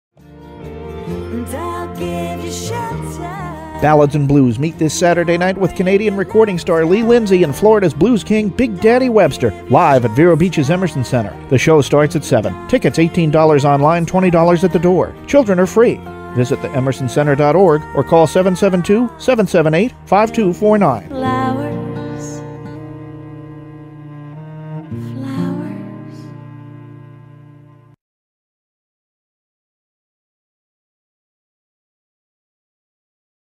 Radio Promo - Vero Beach Emerson Center, FL
Lee Lindsey & Randy "Big Daddy" Webster team up to do a show at The Emerson Center in Vero Beach on Saturday November 16th 2013!